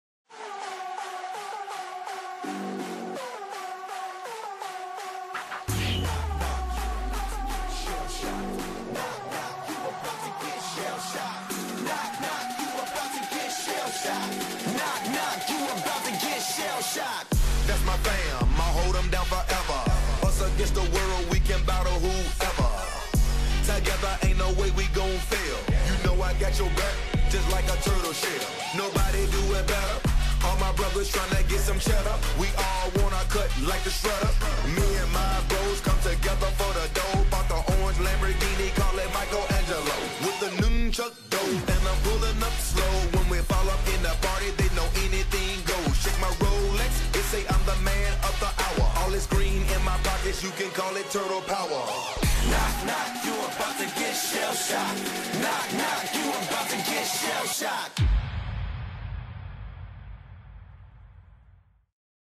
Teenage Mutant Ninja Turtles Time-lapse Chalk Mural
TMNT Time-lapse video of a Chalk Mural created by Cecelia Linayao and inspired by the movie, in theaters August 8.